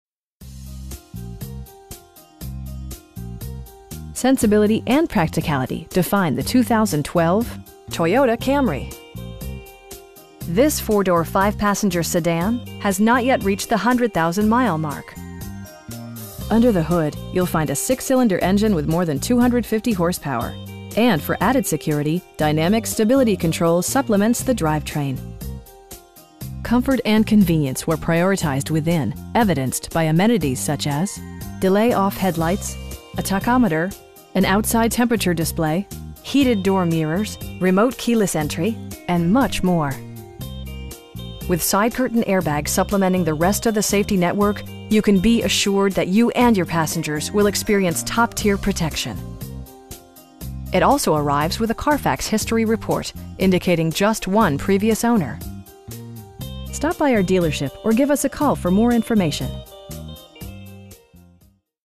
2012 Toyota Camry SE in Conroe, TX 77304
Sensibility and practicality define the 2012 Toyota Camry. This 4 door, 5 passenger sedan has not yet reached the hundred thousand mile mark! Under the hood you'll find a 6 cylinder engine with more than 250 horsepower, and for added security, dynamic Stability Control supplements the drivetrain. Comfort and convenience were prioritized within, evidenced by amenities such as: delay-off headlights, a tachometer, an outside temperature display, heated door mirrors, remote keyless entry, an overhead console, and much more. With side curtain airbags supplementing the rest of the safety network, you can be assured that you and your passengers will experience top-tier protection. It also arrives with a Carfax history report, indicating just one previous owner. Our aim is to provide our customers with the best prices and service at all times. Stop by our dealership or give us a call for more information.